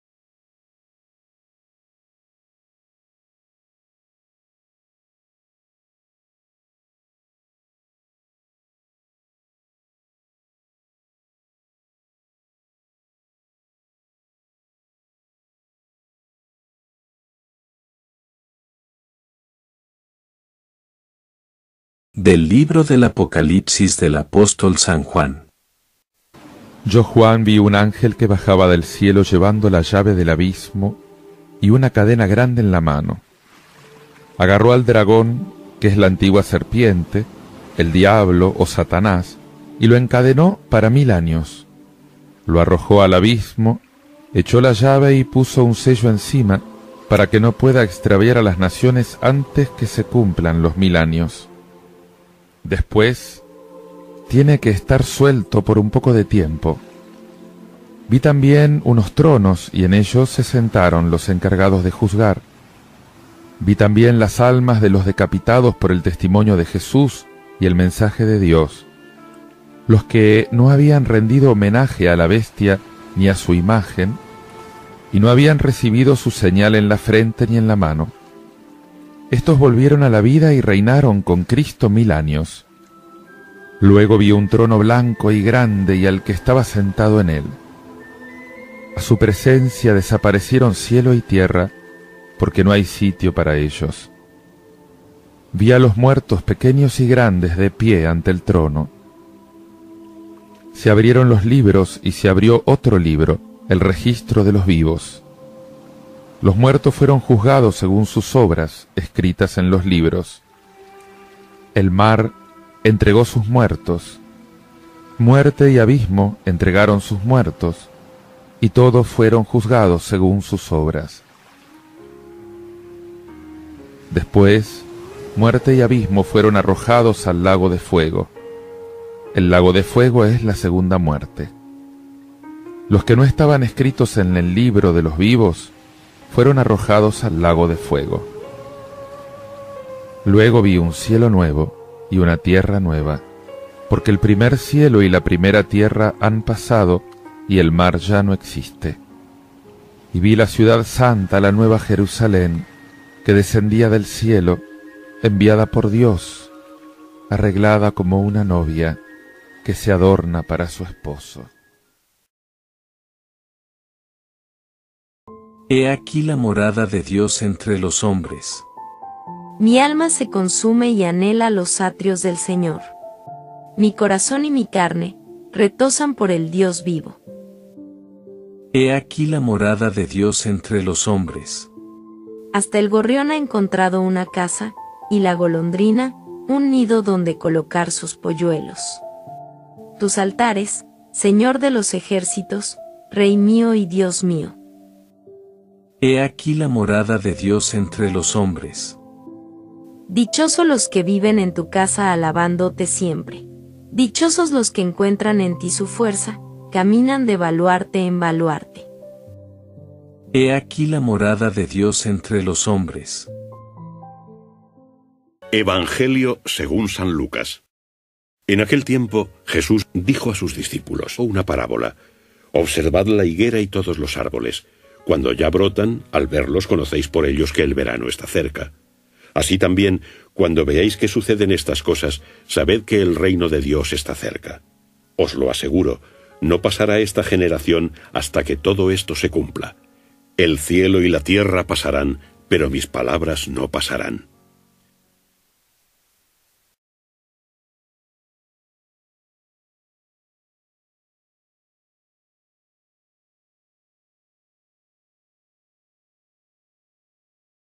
EVANGELIO LECTURAS VIERNES 25 DE NOVIEMBRE DE 2022 | LUCAS 21,29-33
EVANGELIO Y LECTURAS EUCARISTÍA VIERNES 25 DE NOVIEMBRE DE 2022 VIERNES XXXIV TRIGÉSIMA CUARTA SEMANA DEL TIEMPO ORDINARIO Primera Lectura: Del libro del Apocalipsis 20, 1-4. 11–21, 2 Salmo 83,3.4.5-6a y 8a. R/. He aquí la morada de Dios entre los hombres EVANGELIO: San Lucas 21, 29-33